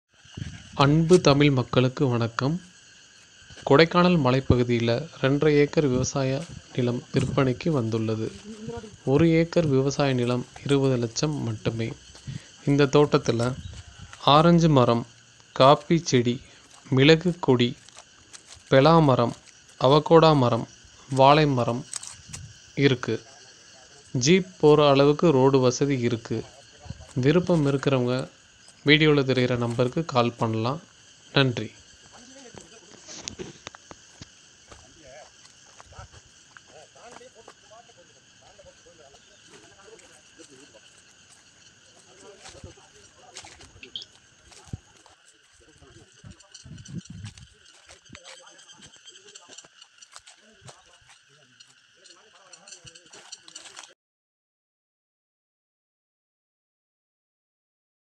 2 1/2 ஏக்கர் விவசாய நிலம் விற்பனைக்கு உள்ளது. காபி, மிளகு, ஆரஞ்சு, வாழை, தண்ணீர் வசதி உள்ளது.
2 1/2 ஏக்கர் விவசாய நிலம் விற்பனைக்கு உள்ளது.
1 ஏக்கர் 20லட்சம் மட்டுமே